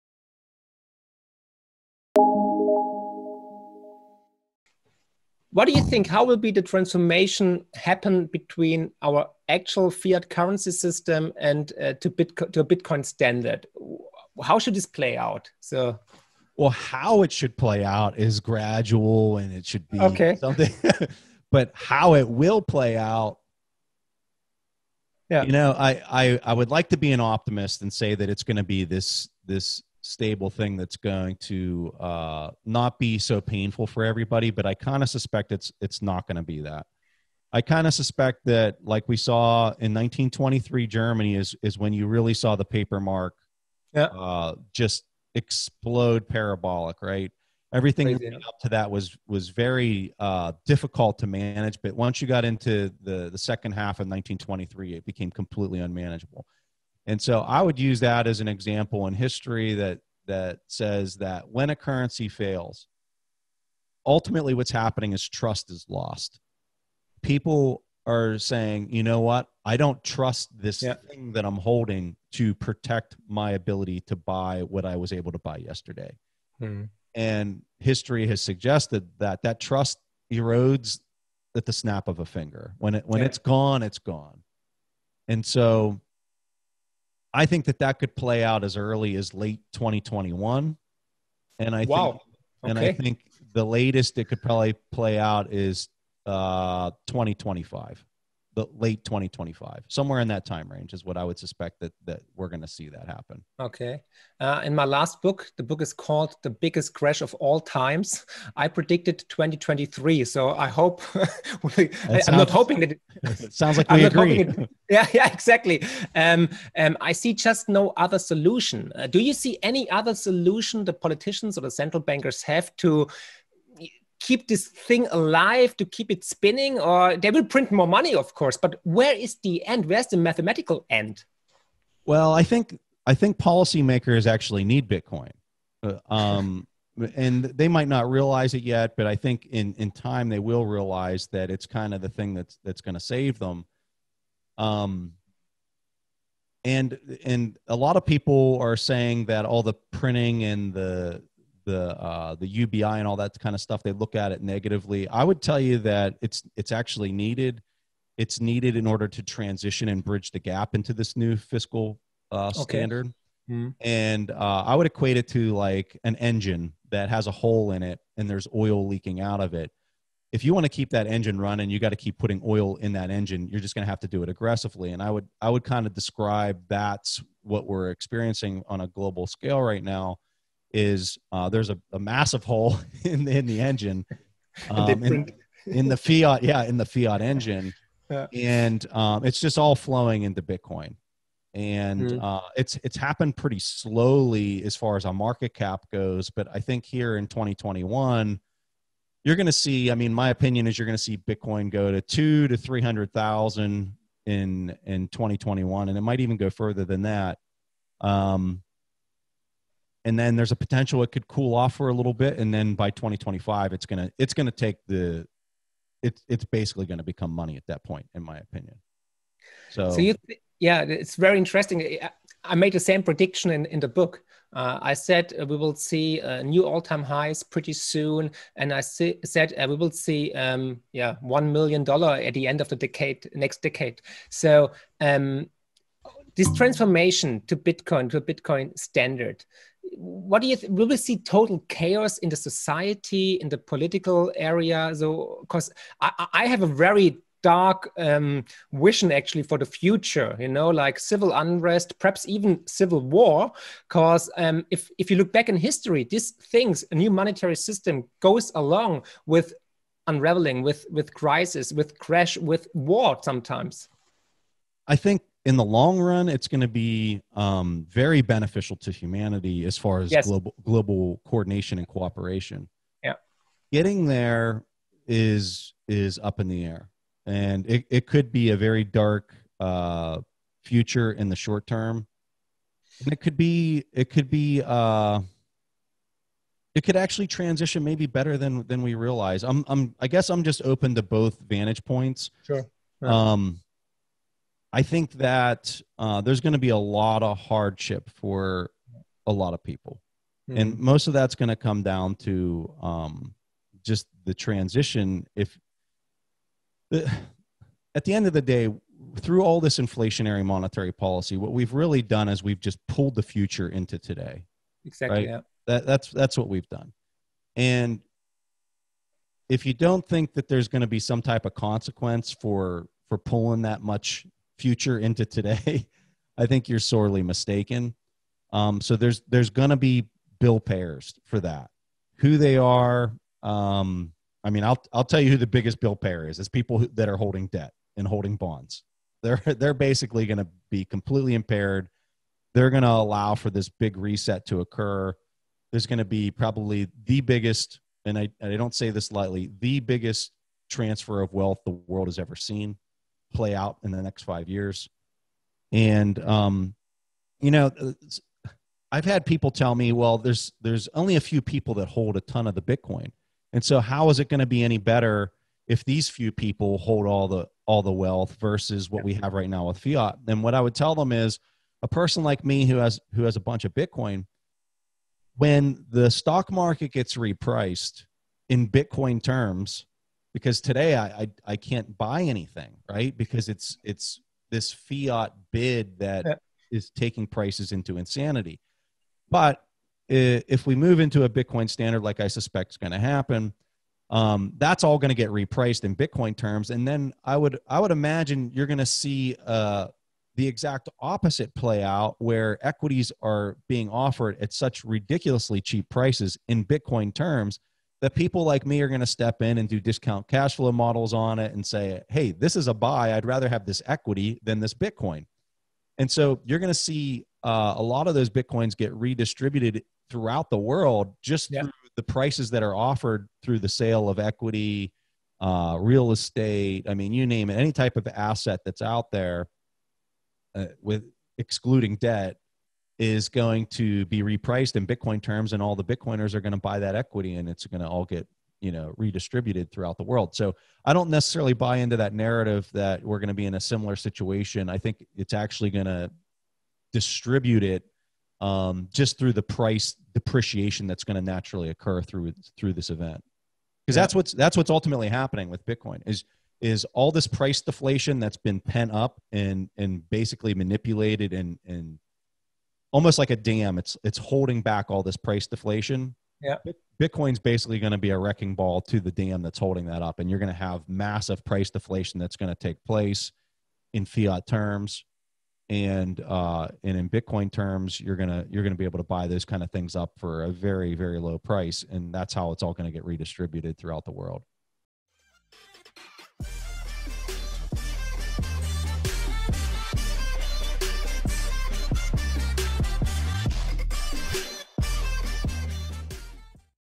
비트코인 본위제는 어떻게 오게 될까? (Feat. 프레스턴 피쉬, pt2)
경제/투자관련 팟캐스트 진행자로 유명한 프레스턴 피쉬 (Preston Pysh)와 독일의 파이낸스 유튜버 마크 프리드리히(Marc Friedrich)의 대화 파트 2입니다.

이 영상에서는 초인플레이션에 대해서 이야기 했던 파트 1 영상에 이어서 비트코인 본위제로 넘어가는 과정에 대해서 이야기합니다.

여러분의 생각은 어떠신지 댓글로 꼭 남겨주세요.

주변 지인들과도 많이 공유해주세요.
이 영상은 투자조언이 아닙니다.
모든 투자의 책임은 본인에게 있습니다.